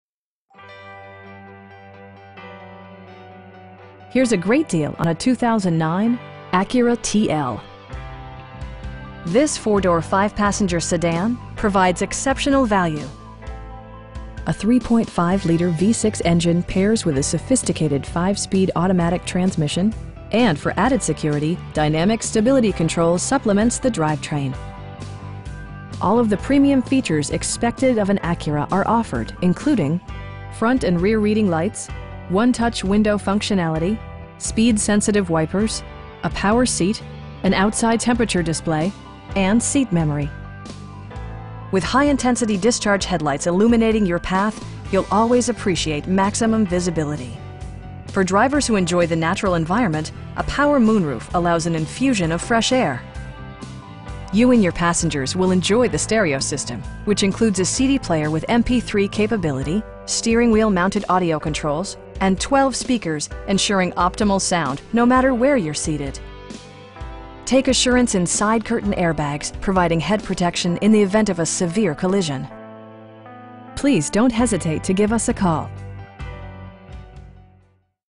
2009 Acura TL 3.5 w/Technology Package in Concord, NC 28027
Here's a great deal on a 2009 Acura TL. This 4 door, 5 passenger sedan provides exceptional value! A 3.5 liter V-6 engine pairs with a sophisticated 5 speed automatic transmission, and for added security, dynamic Stability Control supplements the drivetrain. All of the premium features expected of an Acura are offered, including: front and rear reading lights, 1-touch window functionality, speed sensitive wipers, a power seat, an outside temperature display, an overhead console, and seat memory. With high intensity discharge headlights illuminating your path, you'll always appreciate maximum visibility. For drivers who enjoy the natural environment, a power moon roof allows an infusion of fresh air. You and your passengers will enjoy the stereo system, which includes a CD player with MP3 capability, steering wheel mounted audio controls, and 12 speakers, providing excellent sound throughout the cabin. Take assurance in side curtain airbags, providing head protection in the event of a severe collision. We have the vehicle you've been searching for at a price you can afford. Please don't hesitate to give us a call.